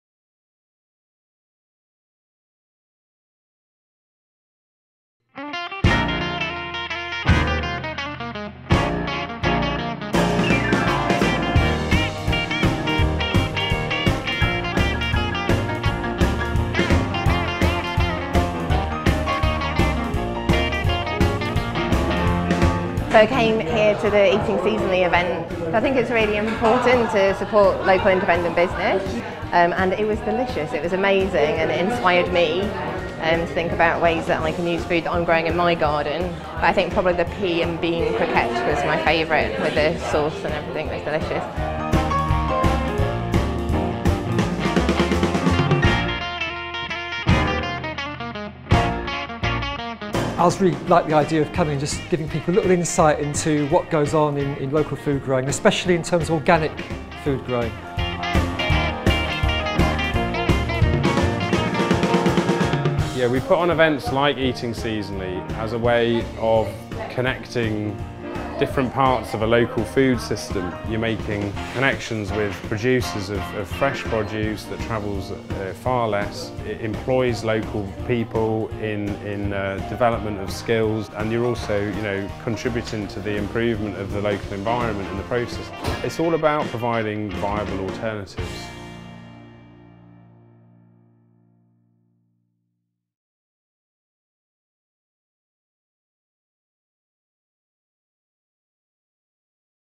Sheffield Co-Op Regather promotes local growers at Eating Seasonally
Sheffield Unchained had the tantalising privilege of attending Regather’s second Eating Seasonally Supper Club, where we spoke to one of the co operative’s founders Gareth Roberts and had a very tasty supper.

Written & Directed by Joe & Marishka Bream
Filmed by Joe Bream
Edited by Joe & Marishka Bream

This is a Humbledinger film for Sheffield Unchained, you can see more Humbledinger films